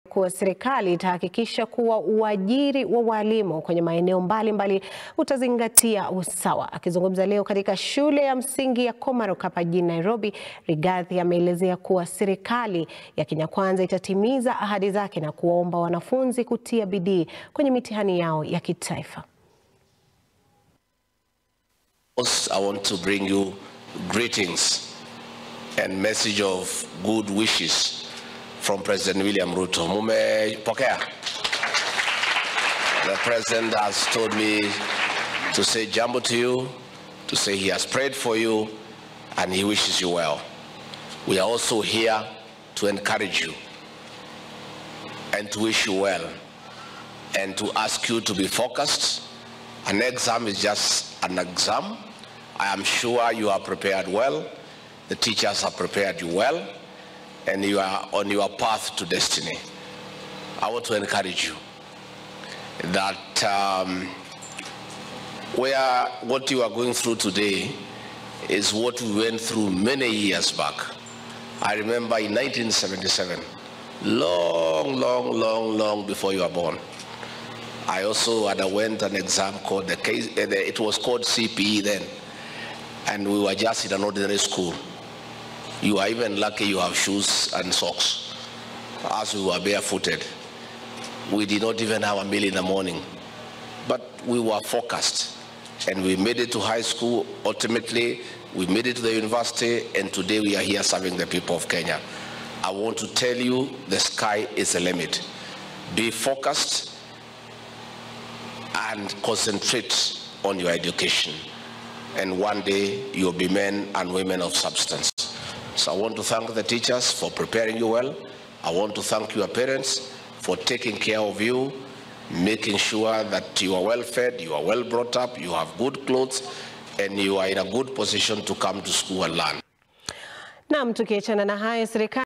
KCPE na KPSEA siku ya pili
Naibu wa Rais awatakia  wanafunzi kila la kheri  kwenye mitihani yao.